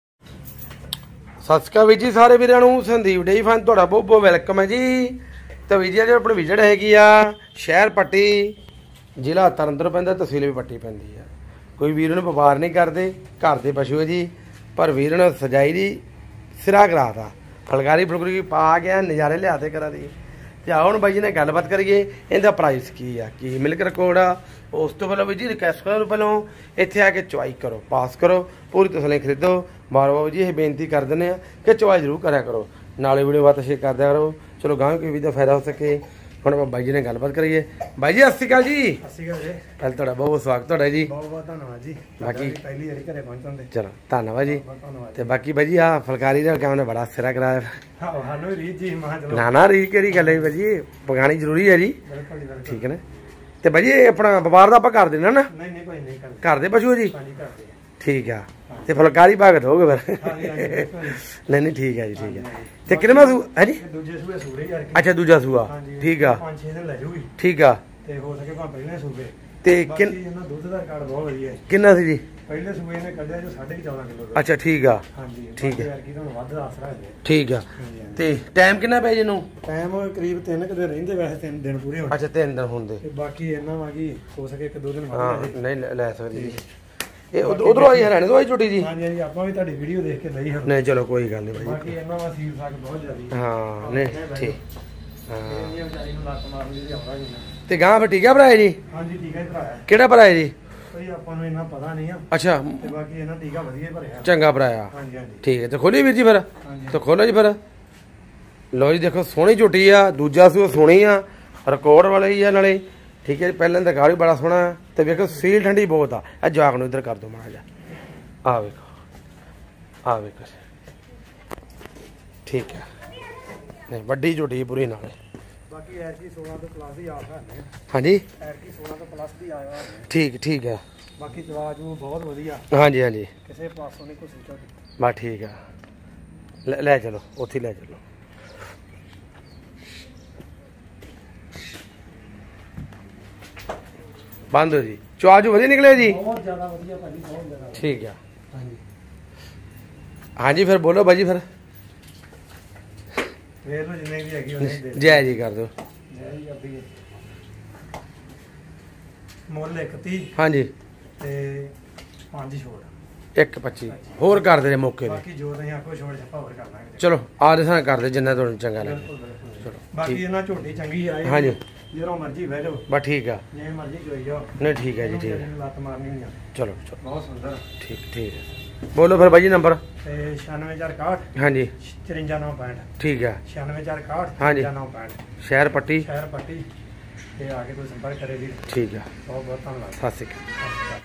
29 September 2023 ਫੋਨ-96461-54965 ਪੱਟੀ ਮੌੜ ਪਿੰਡ ਆ ਬਾਈ ਦਾ ਇਹਦੀ ਮੱਝ 16 ਕਿਲੋ ਦੁੱਧ ਆਲੀ ਦੇਖਣ ਨੂੰ ਈ ਪਤਾ।
29 September 2023 ਫੋਨ-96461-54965 ਪੱਟੀ ਮੌੜ ਪਿੰਡ ਆ ਬਾਈ ਦਾ ਇਹਦੀ ਮੱਝ 16 ਕਿਲੋ ਦੁੱਧ ਆਲੀ ਦੇਖਣ ਨੂੰ ਈ ਪਤਾ।